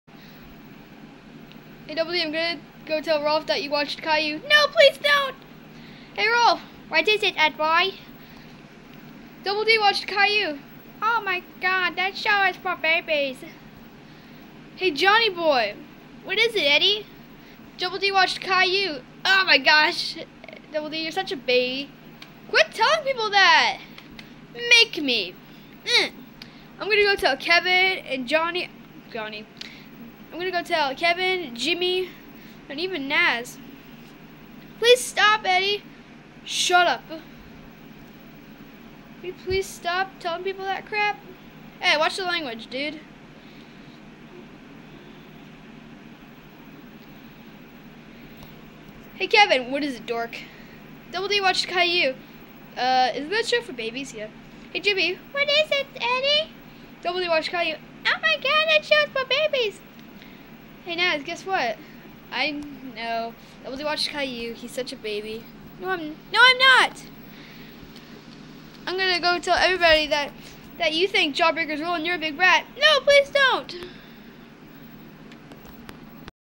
Ed Edd n Eddy 3D Episode 8: Eddy Tells People That Edd Watched Caillou
when Double D told Ed and Eddy that he watched caillou he told all the kids!